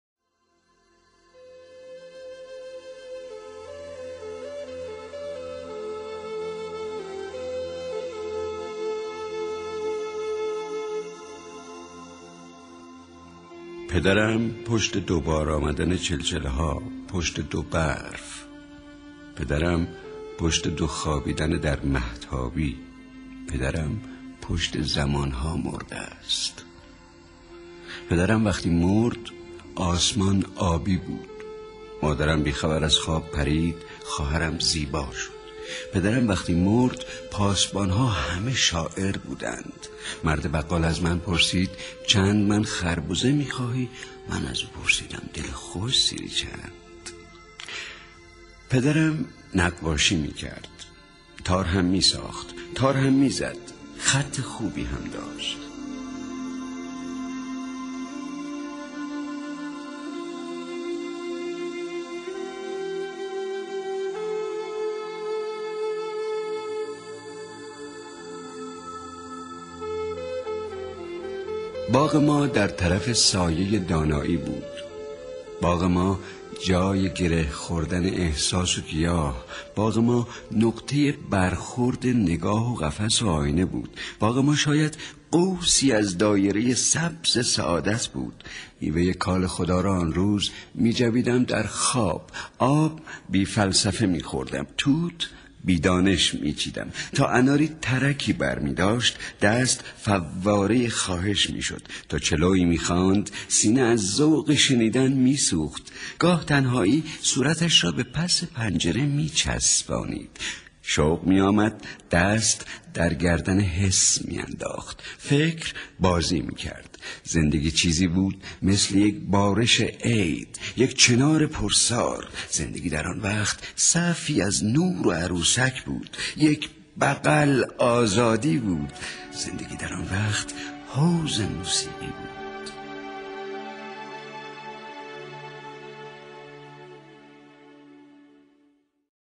Sohrab Sepehri - Sedaye Paye Ab - Part 2/9
Sohrab Sepehri (Persian/Iranian Poet) - Sedaye Paye Ab (The sound of the footsteps of water) - Voice: Khosro Shakibaee; Video and Photo Editing: Pirouz Ebadypour